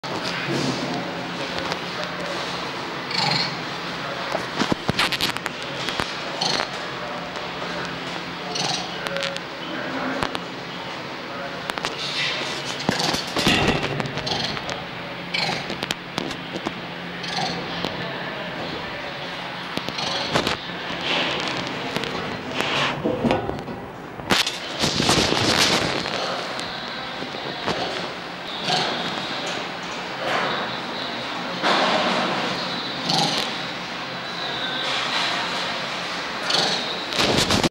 Affenhölle Covance
Dutzende Affen kamen per Lufttransport in das Labor. Die Augen der verängstigten sprachen für sich.

copyright:Mülln/buav